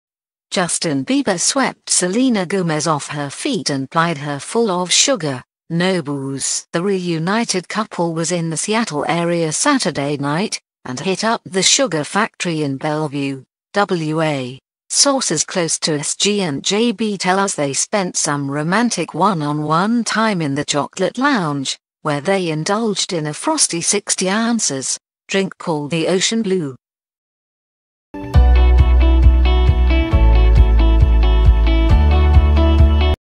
JUSTIN BIEBER & SELENA GOMEZ Flew to Seattle for SWEETEST 'VIRGIN' DATE | HUX
JUSTIN BIEBER & SELENA GOMEZ Flew to Seattle for SWEETEST 'VIRGIN' DATE | HUX Justin Bieber swept Selena Gomez off her feet and plied her full of sugar ... no booze.

The reunited couple was in the Seattle area Saturday night, and hit up the Sugar Factory in Bellevue, WA. Sources close to SG and JB tell us they spent some romantic one-on-one time in the Chocolate Lounge, where they indulged in a frosty 60 oz. drink called the Ocean Blue.

Holywod updates X.